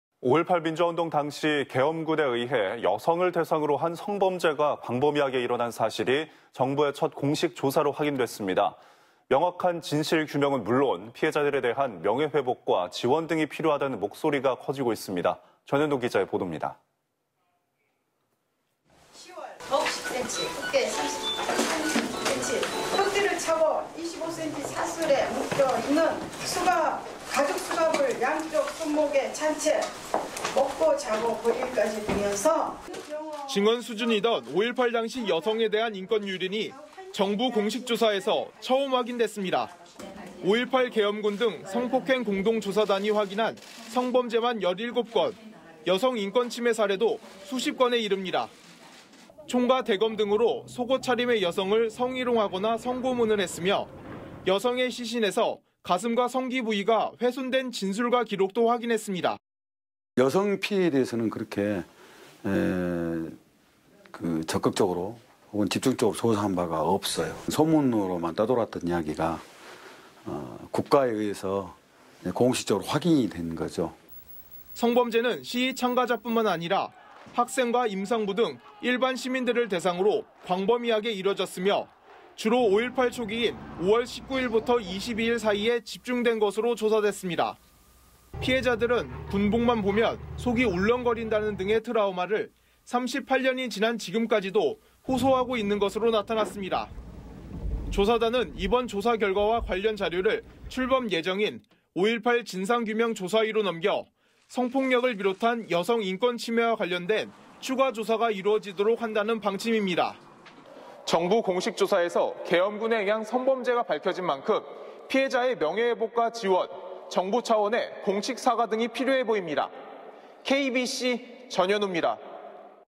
5.18 계엄군에 짓밟힌 삶..38년 지나도록 고통 여전
5.18민주화운동 당시 계엄군에 의해 여성을 대상으로 한 성범죄가 광범위하게 일어난 사실이 정부의 첫 공식 조사로 확인됐습니다.

명확한 진실 규명은 물론, 피해자들에 대한 명예회복과 지원 등이 필요하다는 목소리가 커지고 있습니다.

전현우 기자의 보돕니다.